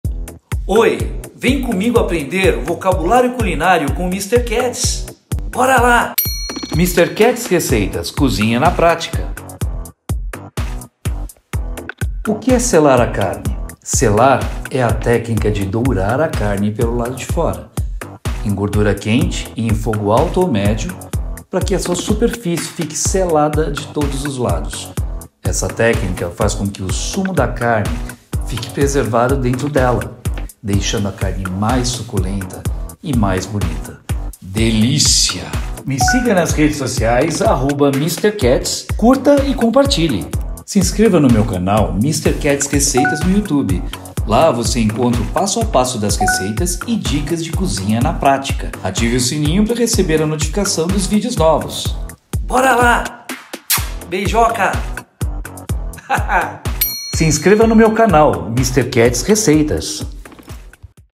VOCABULÁRIO CULINÁRIO: O QUE É SELAR? | by Mister Kats Receitas - Cozinha na Prática
O que é selar a carne? Selar é a técnica de dourar a carne pelo lado de fora. Em gordura quente e em fogo alto ou médio para que a sua superfície fique selada de todos os lados. Essa técnica faz com que o o sumo da carne fique preservado dentro dela, deixando a carne suculenta e mais bonita. Delícia!

Curta e compartilhe!
Bigaduuuuu!!!